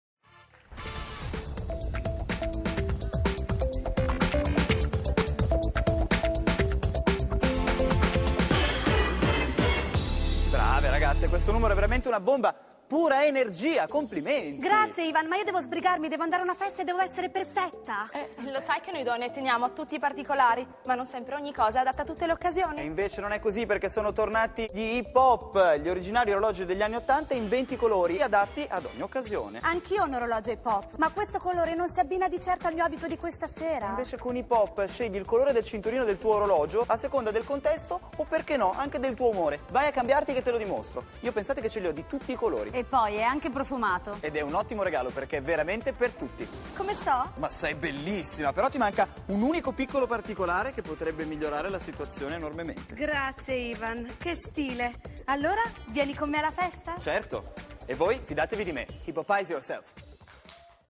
Asd Dlight - pubblicità orologi 2
Telepromozione orologi Hip Hop
Anno 2012, Raidue
in performance Diana Verona, Asd d·Light